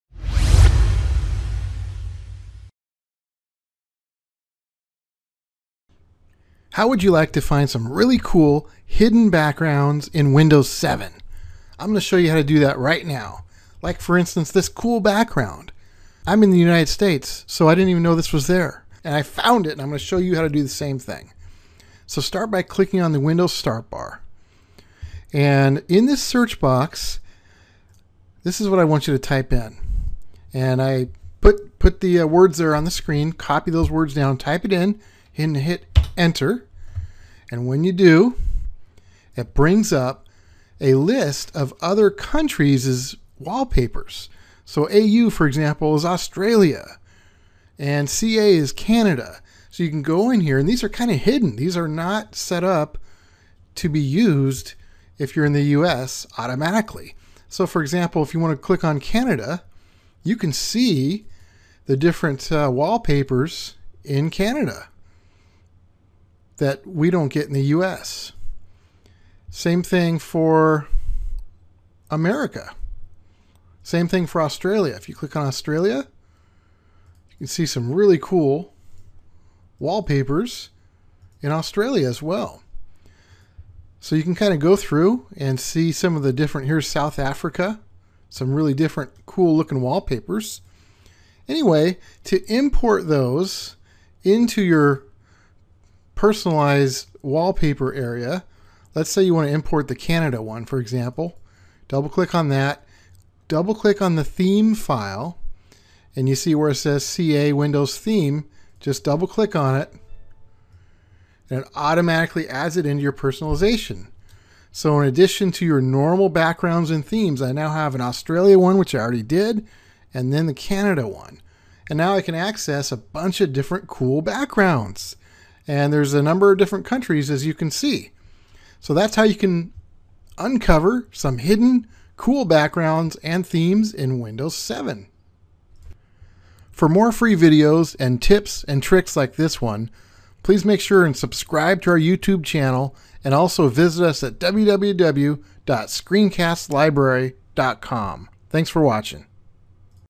How To Find Secret Hidden Backgrounds And Themes In Windows 7
This Windows 7 tips video will show you how to find and uncover some secret, hidden backgrounds and themes in Windows 7 from other Countries! You can use these quickly and easily and they look REALLY cool.